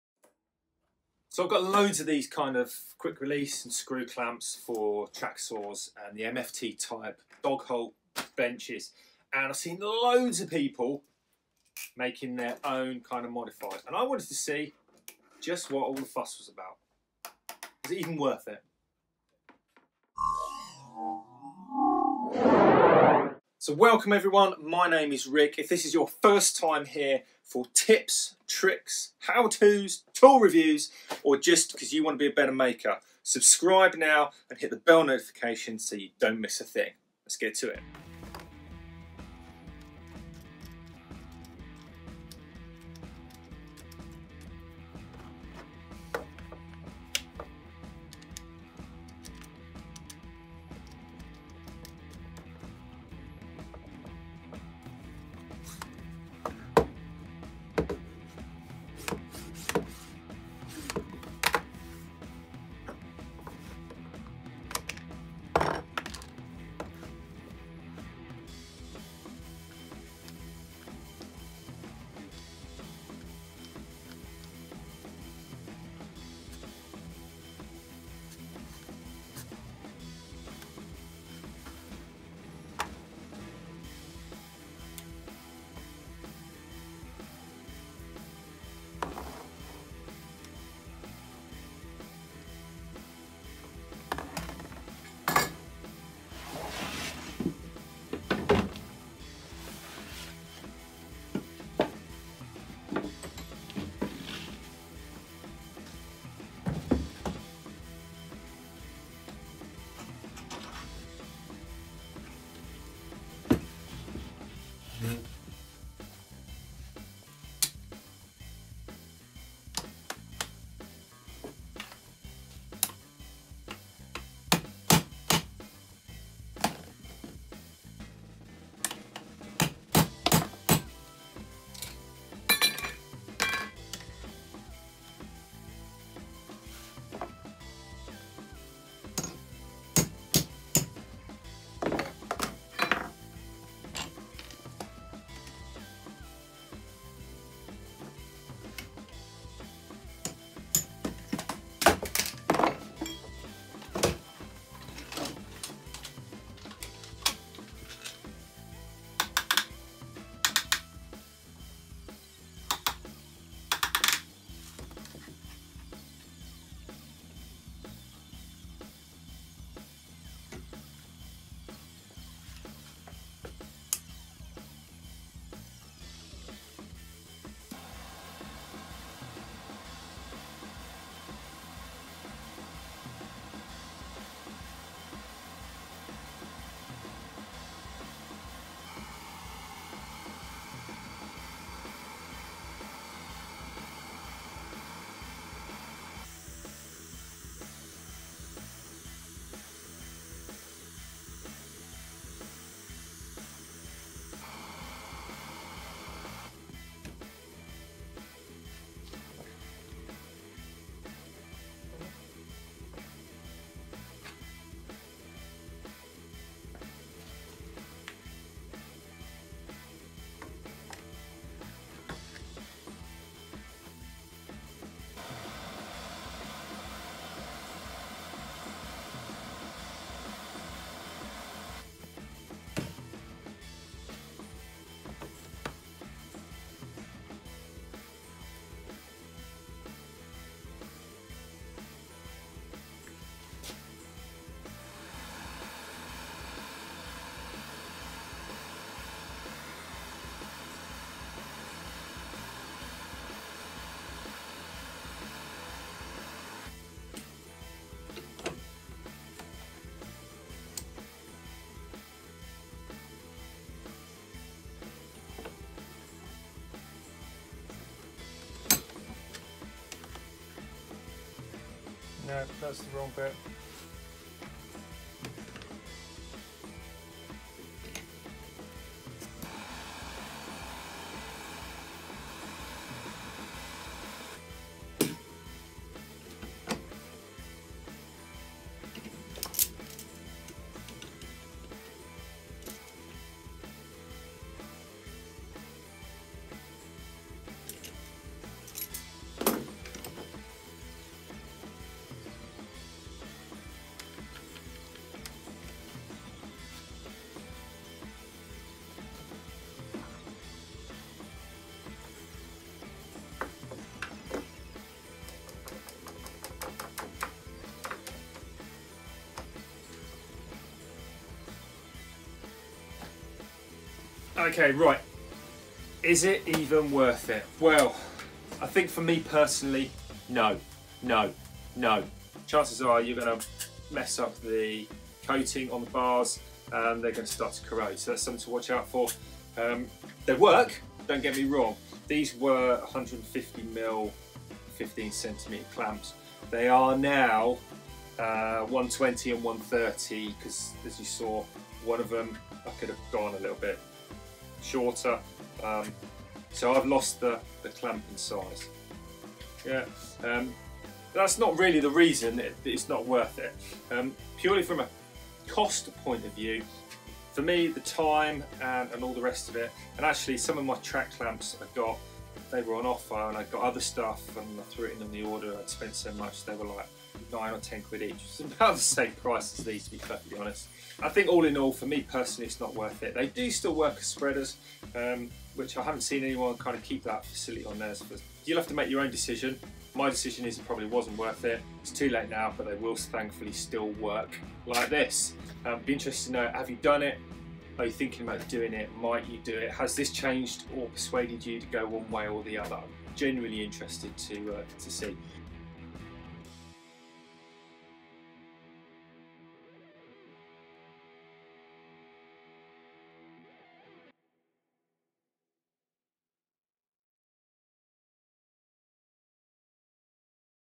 EASY DIY HACK: Modify Quick clamps for use with MFT Workbench or Dog holes
It's really easy and quick to do,  this video shows the entire simple process: but is it worth the effort? or is it a waste of time and money?

I give my thoughts, things i discovered along the way, my views on the outcome, and the reasons why - you'll have to make your own decision!

It might be a useful Mft top clamping option. A trick for site work with an MFT slab. Maybe it would be useful for an cheap addition to your Festool MFT table or as a workmate hack.

You decide.